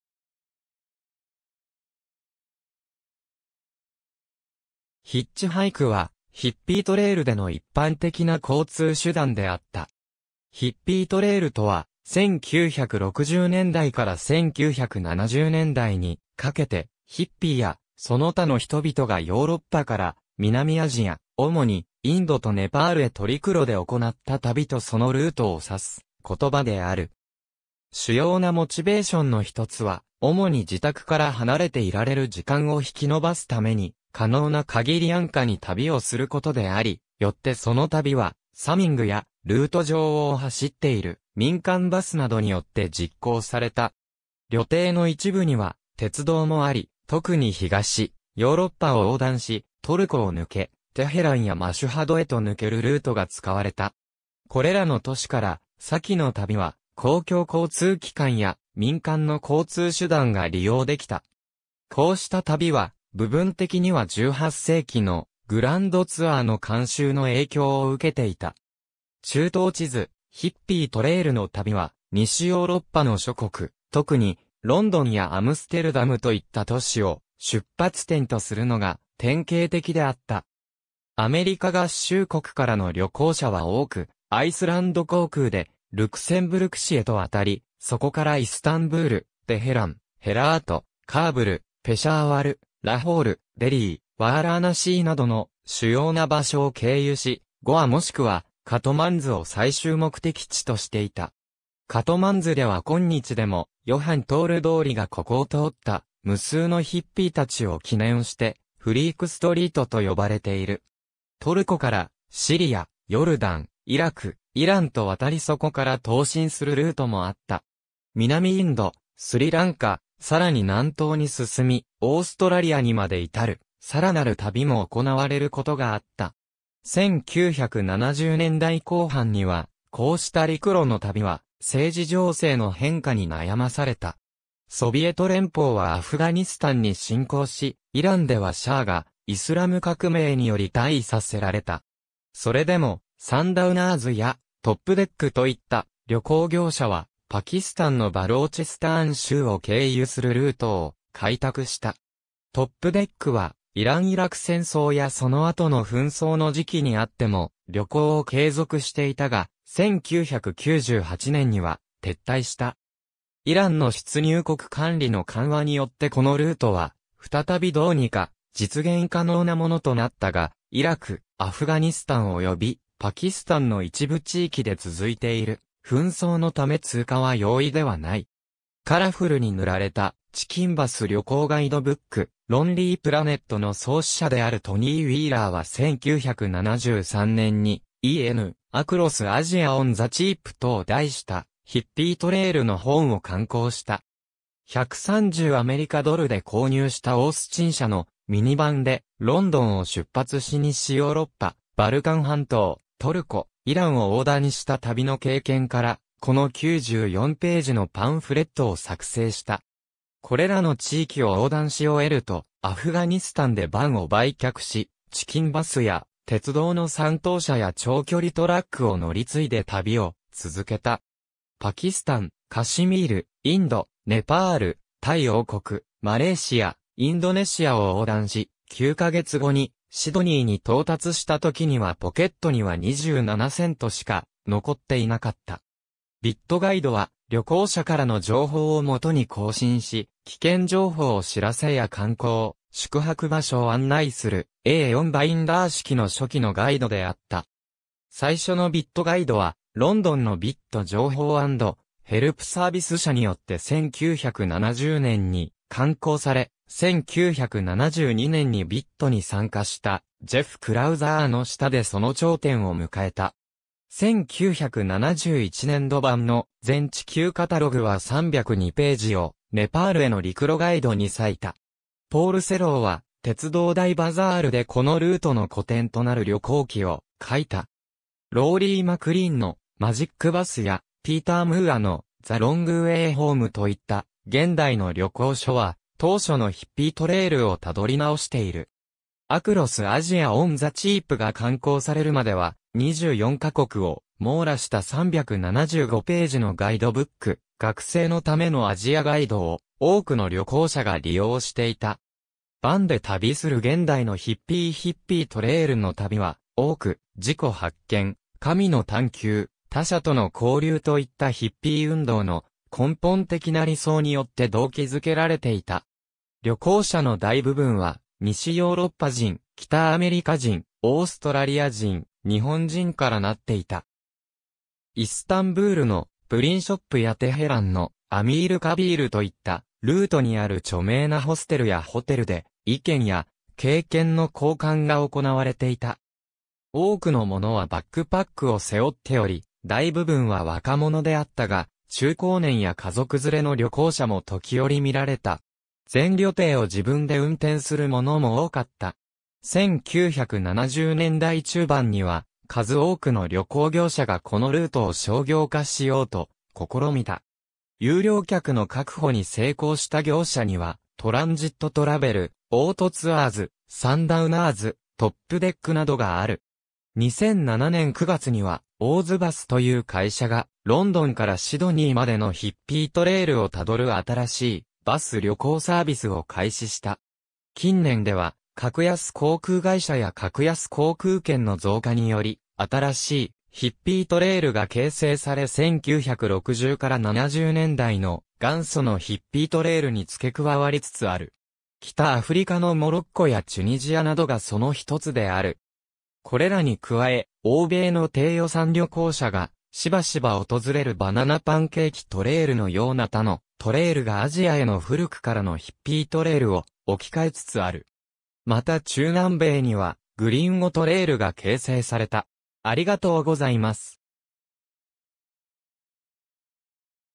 ヒッピー・トレイル
ヒッピー・ムーブメント
ヒッチハイクはヒッピー・トレイルでの一般的な交通手段であった。
ヒッピー・トレイル（英語: hippie trail）とは、1960年代から1970年代にかけてヒッピーやその他の人々がヨーロッパから南アジア、主にインドとネパールへと陸路で行った旅とそのルートを指す言葉である。
主要なモチベーションの1つは、主に自宅から離れていられる時間を引き延ばすために、可能な限り安価に旅をすることであり、よってその旅は「サミング」（ヒッチハイク）や、ルート上を走っている民間バスなどによって実行された。
旅程の一部には鉄道もあり、特に東ヨーロッパを横断し（ヴァン湖をフェリーで渡り）トルコを抜け、テヘランやマシュハドへと抜けるルートが使われた。
これらの都市から先の旅は、公共交通機関や民間の交通手段が利用できた。
こうした旅は、部分的には18世紀の「グランドツアー」の慣習の影響を受けていた。
中東地図 ヒッピー・トレイルの旅は西ヨーロッパの諸国、特にロンドンやアムステルダムといった都市を出発点とするのが典型的であった。
アメリカ合衆国からの旅行者は多くアイスランド航空でルクセンブルク市へと渡り、そこからイスタンブール、テヘラン、ヘラート、カーブル、ペシャーワル、ラホール、デリー、ワーラーナシー（当時ベナレスと呼ばれていた）などの「主要な」場所を経由し、ゴアもしくはカトマンズを最終目的地としていた。
カトマンズでは今日でもJochen Tole通りがここを通った無数のヒッピーたちを記念して「フリーク・ストリート」と呼ばれている。
トルコからシリア、ヨルダン、イラク、イランと渡りそこから東進するルートもあった。
南インド、スリランカ（当時セイロン）、さらに南東に進みオーストラリアにまで至るさらなる旅も行われることがあった。
1970年代後半にはこうした陸路の旅は政治情勢の変化に悩まされた。
ソビエト連邦はアフガニスタンに侵攻し、イランではシャーがイスラム革命により退位させられた。
それでも、「サンダウナーズ」や「トップデック」（en:Topdeck）といった旅行業者はパキスタンのバローチスターン州を経由するルートを開拓した。
トップデックはイラン・イラク戦争やその後の紛争の時期にあっても旅行を継続していたが、1998年には撤退した。
イランの出入国管理の緩和によってこのルートは再びどうにか実現可能なものとなったが、イラク、アフガニスタン及びパキスタンの一部地域で続いている紛争のため通過は容易ではない。
カラフルに塗られた「チキン・バス」（写真はグアテマラのもの） 旅行ガイドブック『ロンリープラネット』の創始者であるトニー・ウィーラーは1973年に『en:Across Asia On The Cheap』（安上がりにアジア横断）と題したヒッピー・トレイルの本を刊行した。
130アメリカドルで購入したオースチン社のミニバンでロンドンを出発し西ヨーロッパ、バルカン半島、トルコ、イランを横断した旅の経験から、この94ページのパンフレットを作成した。
これらの地域を横断し終えると、アフガニスタンでバンを売却し、チキン・バスや鉄道の三等車や長距離トラックを乗り継いで旅を続けた。
パキスタン、カシミール、インド、ネパール、タイ王国、マレーシア、インドネシアを横断し、9ヶ月後にシドニーに到達した時にはポケットには27セントしか残っていなかった。
『BIT Guide』は旅行者からの情報を基に更新し危険情報を知らせや観光・宿泊場所を案内するA4バインダー式の初期のガイドであった。
最初のBITガイドはロンドンのBIT情報&ヘルプサービス社によって1970年に刊行され、1972年にBITに参加したジェフ・クラウザーの下でその頂点を迎えた。
1971年度版の『全地球カタログ』（「Whole Earth Catalog最終版」）は302ページを「ネパールへの陸路ガイド」に割いた。
ポール・セローは『鉄道大バザール』（en:The Great Railway Bazaar, 1975）でこのルートの古典となる旅行記を書いた。
ローリー・マクリーンの『マジック・バス』(2008)やピーター・ムーアの『The Wrong Way Home』(1999)といった現代の旅行書は当初のヒッピー・トレイルを辿り直している。
『Across Asia On The Cheap』が刊行されるまでは、24ヶ国を網羅した375ページのガイドブック『学生...